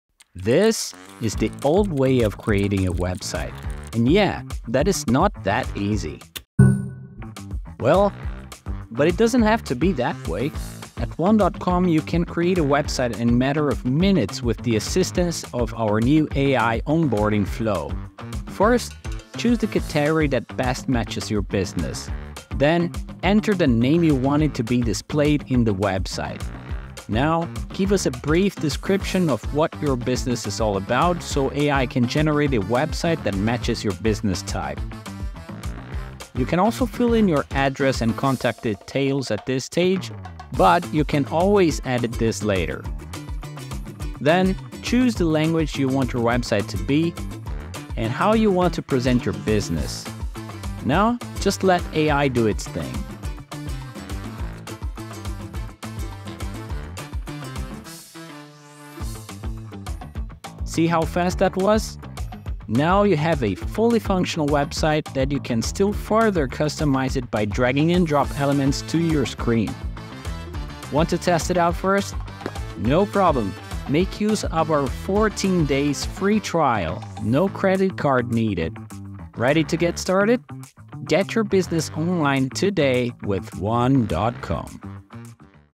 Creating a website with AI | Effortless Website Creation with One.com's AI Onboarding Tool
🚀 Ready to build your own website without breaking a sweat? In this video, we'll guide you through the process of creating a stunning website using the powerful AI Onboarding Tool from one.com.

🛠️ No coding skills? No problem! One.com's AI Onboarding Tool takes the complexity out of website creation, allowing you to focus on what matters most – your content.

Have questions or need assistance? Drop them in the comments below, and we'll be happy to help!

🌐 Ready to get started? Click the link below to sign up with one.com and unlock the potential of AI-assisted website creation!